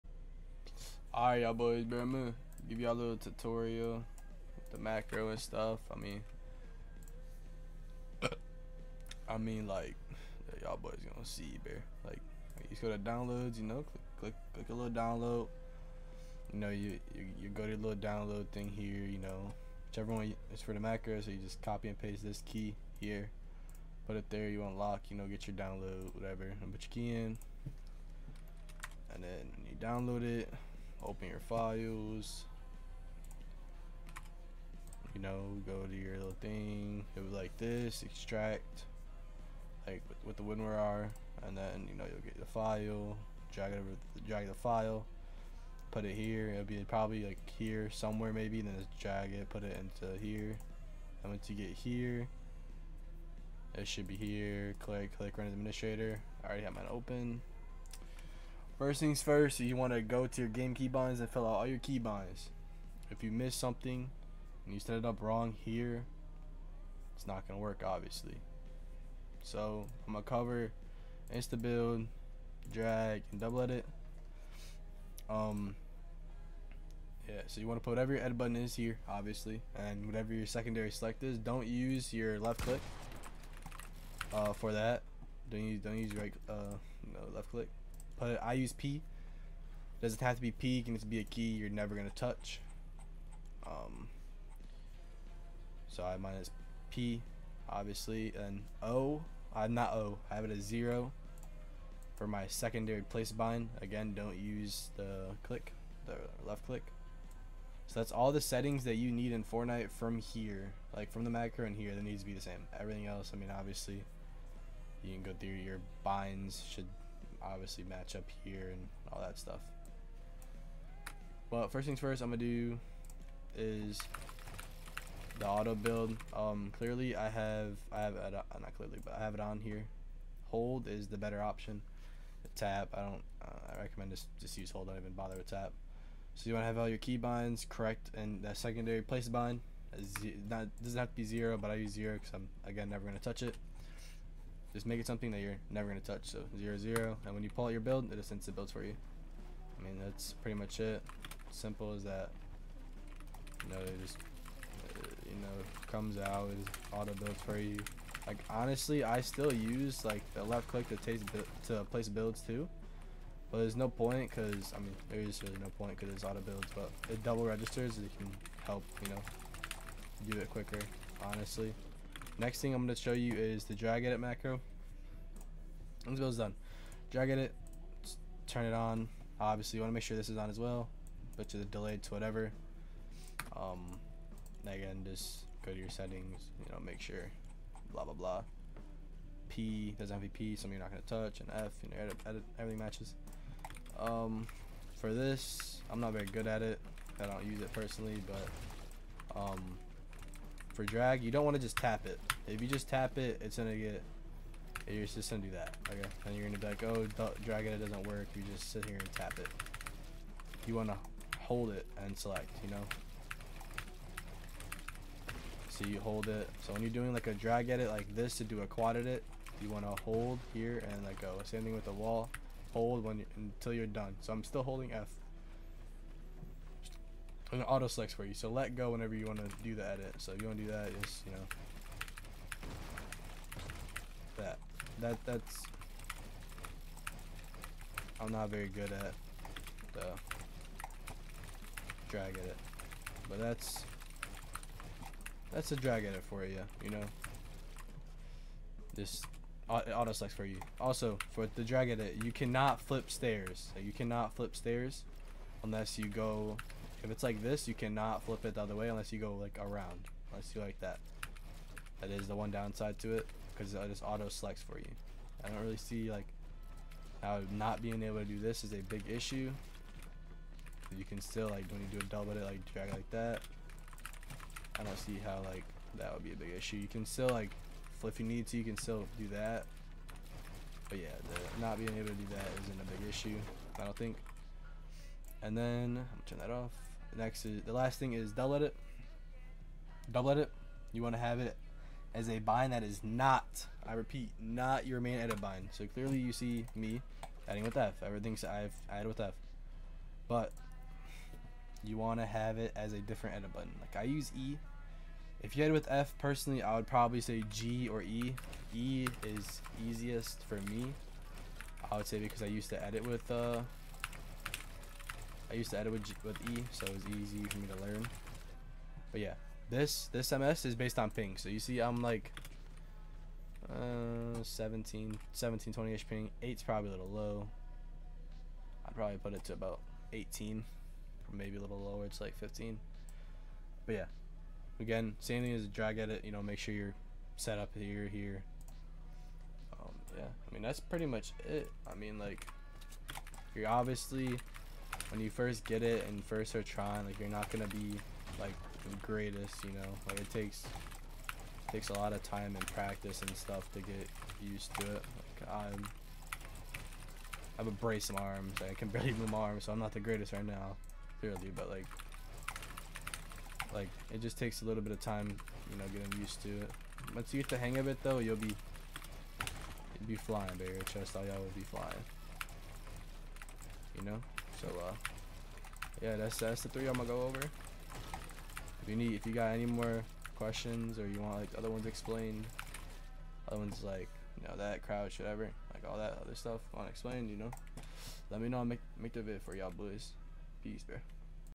Main Macro Tutorial - LLTweaks v1
IN THIS VIDEO I WILL GO OVER THE MAIN MACRO FEATURES.

if yall want a tut on the other features lmk!

Also lmk if you want a controller tut as well